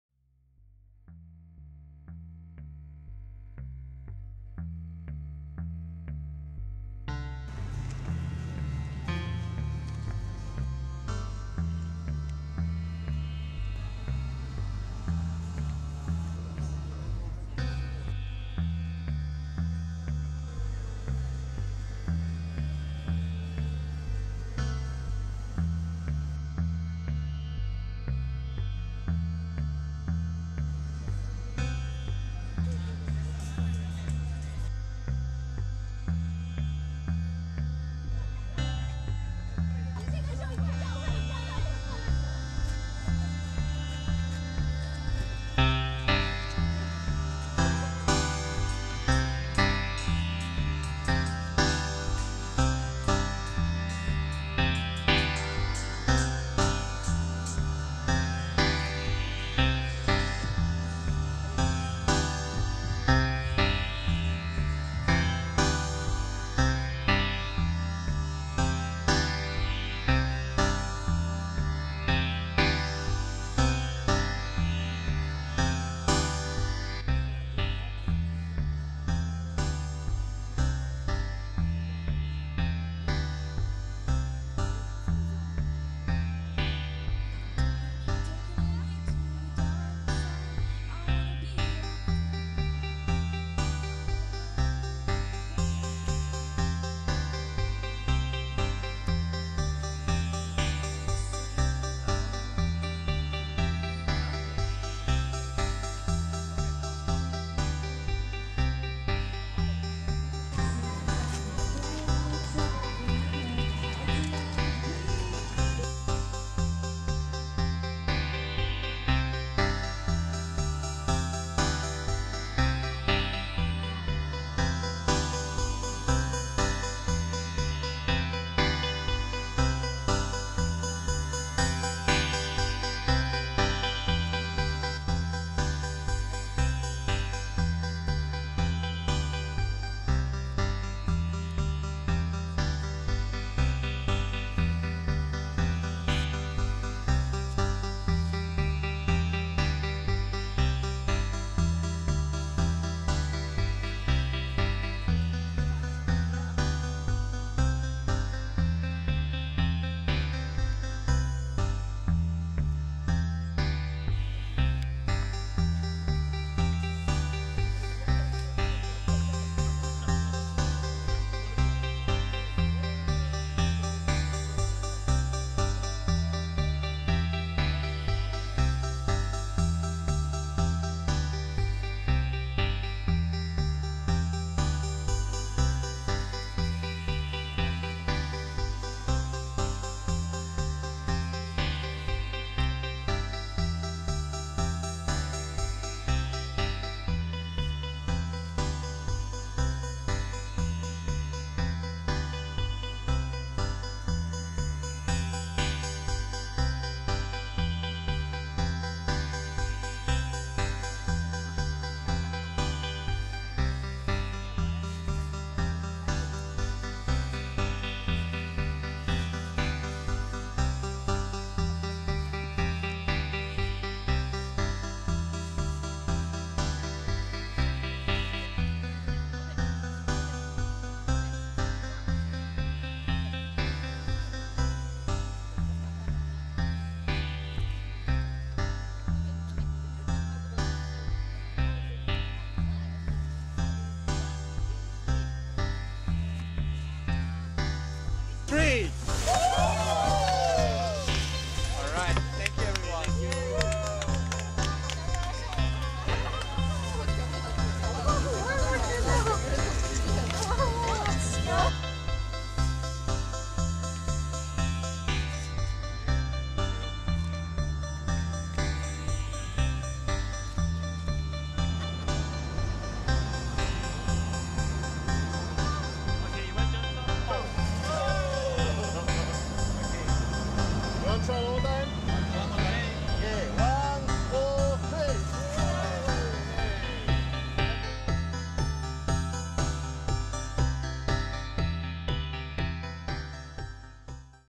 49 Zentai 8 August 2015
49 Zentai

8 August 2015 Saturday
4:30pm-7pm
Bedok Reservoir, Singapore
This is part of PAssionArts Festival 2015

This is Zentai Art Festival Video